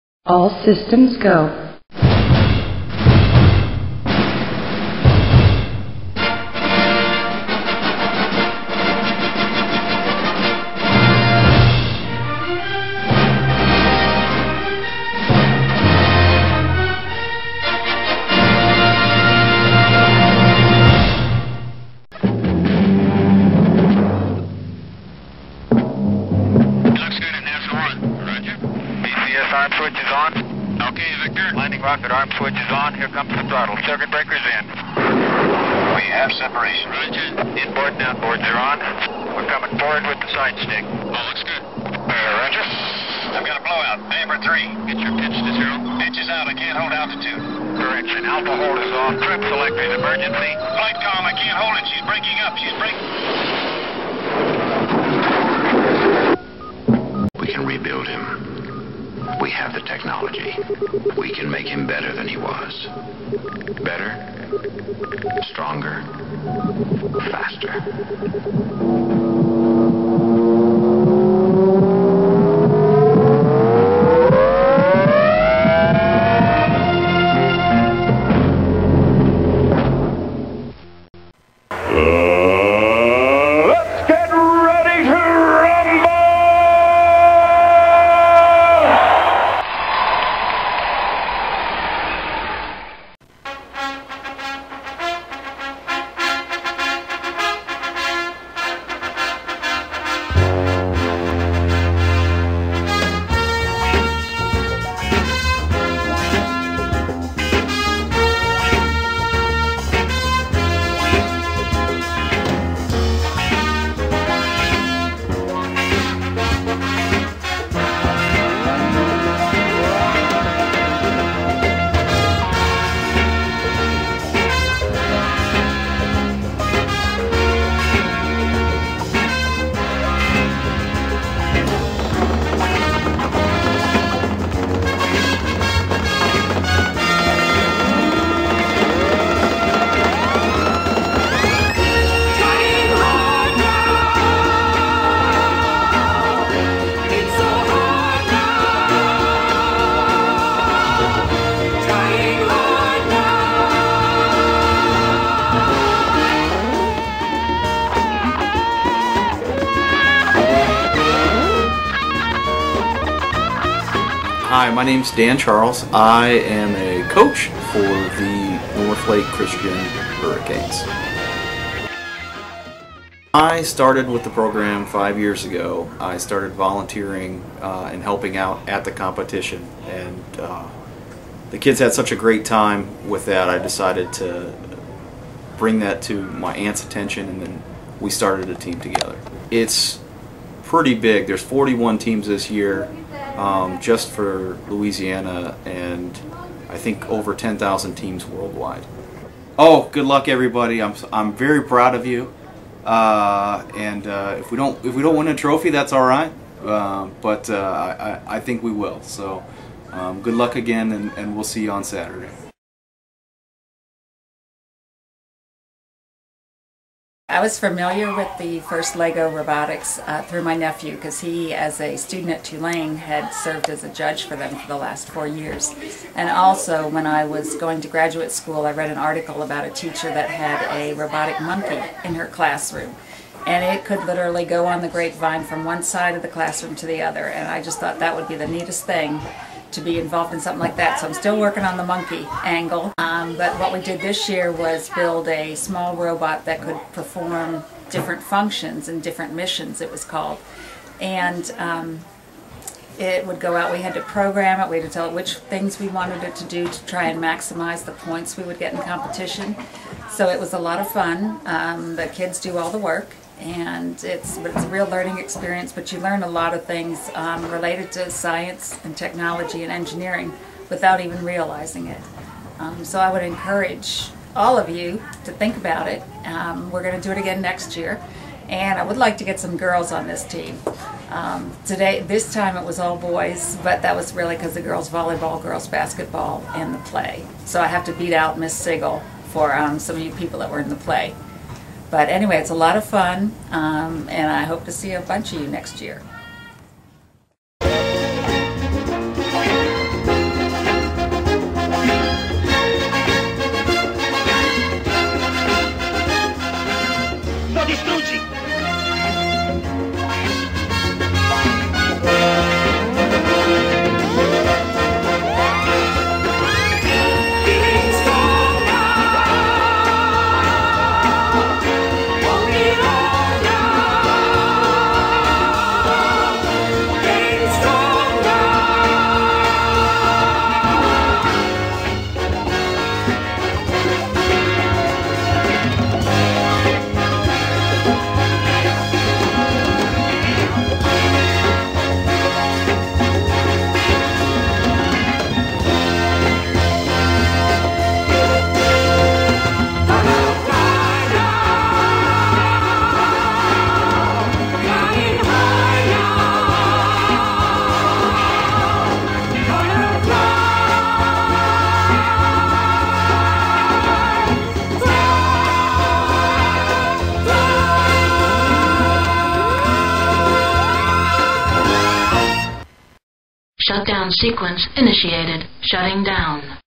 Northlake Christian School Robotics Team
The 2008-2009 NCS Robotics Team, competing in the FIRST LEGO LEAGUE of Louisiana.